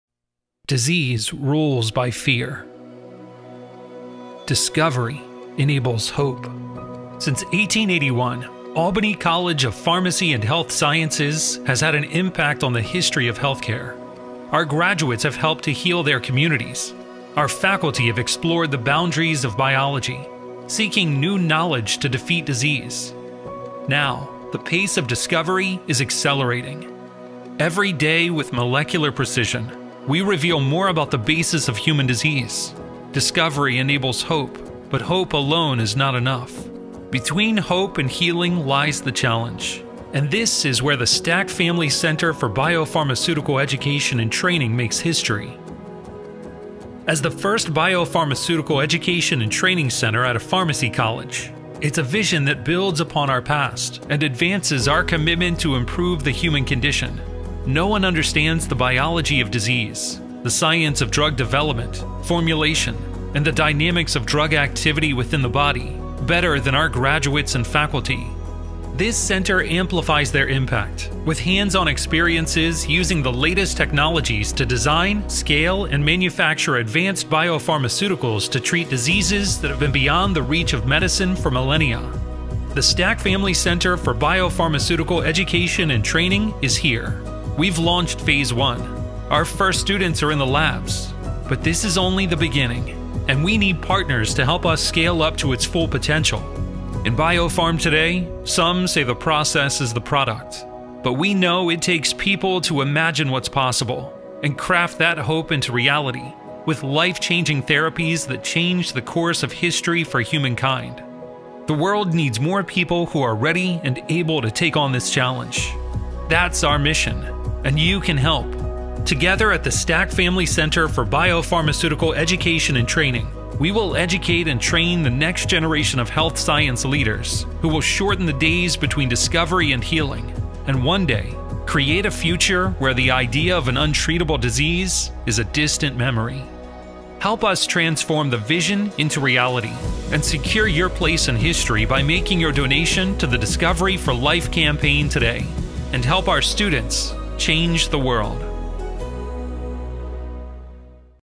Stack Family Center for Biopharmaceutical Education and Training is fueling the future of biopharma.
Stack Family Center for Biopharmaceutical Education and Training (CBET) is a purpose-built bioprocessing center with upstream and downstream laboratories that are equipped with the latest industry-grade technologies, including single-use and traditional systems capable of batch, continuous, and perfusion processes.

Students at CBET will complete hands-on projects across the biomanufacturing continuum, from cell line development, expression system selection, to optimizing media, scaling up to bioreactor inoculation, process monitoring, to collection and downstream purification.